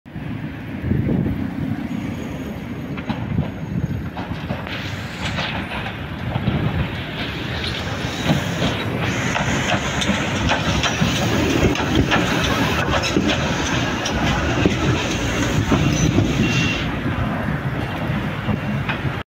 Tramvaiul de școală V3A-93 108 trazitând stația Cimitirul Armenesc
The school tram V3A-93 108 passing in Cimitirul Armenesc tram stop

March 2023